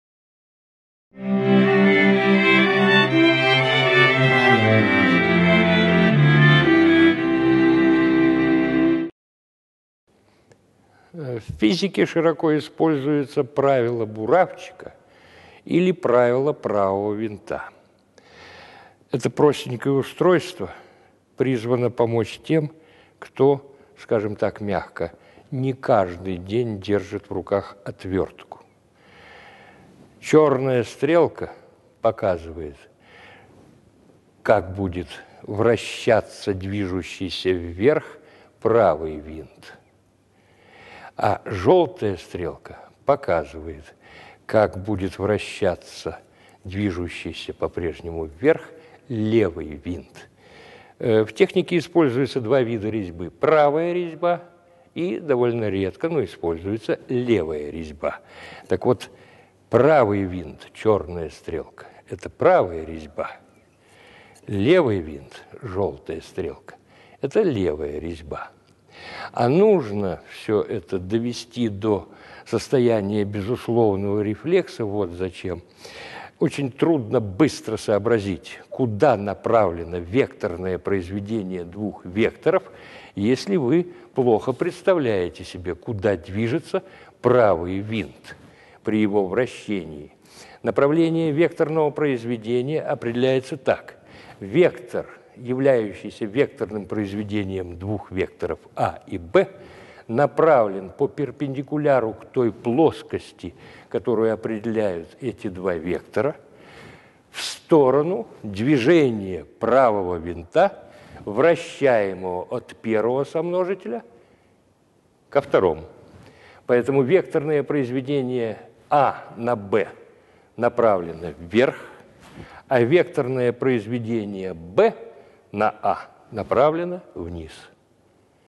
Правое и левое вращение.
Гервидс Валериан Иванович - доцент кафедры общей физики МИФИ, кандидат физико-математических наук.